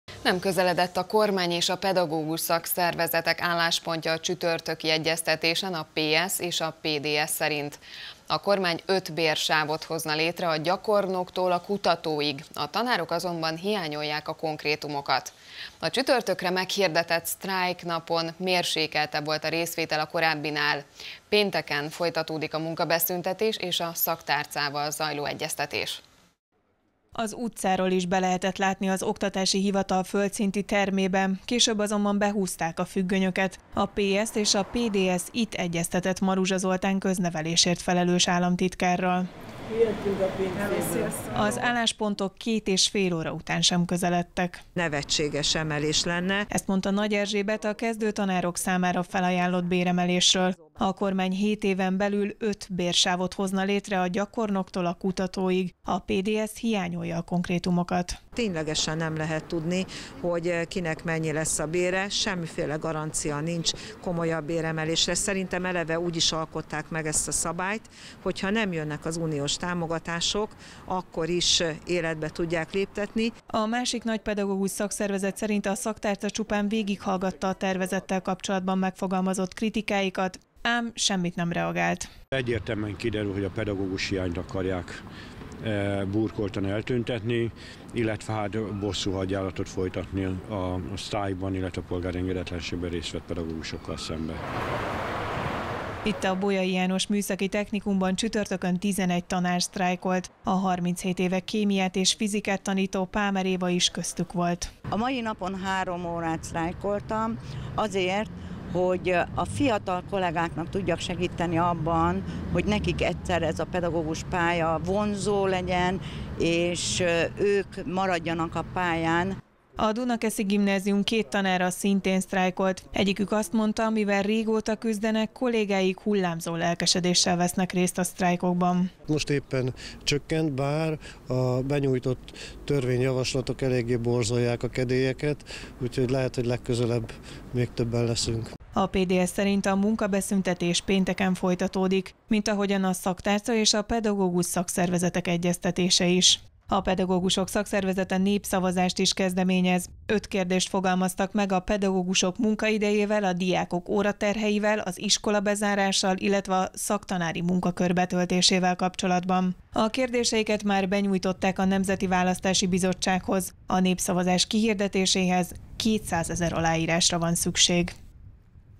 Nem tudott megegyezni a kormány a pedagógusokkal
Nem közeledett a kormány és a Pedagógus Szakszervezetek álláspontja a csütörtöki egyeztetésen a PSZ és PDSZ szerint. A kormány 5 bérsávot hozna létre a gyakornoktól a kutatóig, a tanárok azonban hiányolják a konkrétumokat. A csütörtökre meghirdetett sztrájknapon mérsékeltebb volt a részvétel a korábbinál.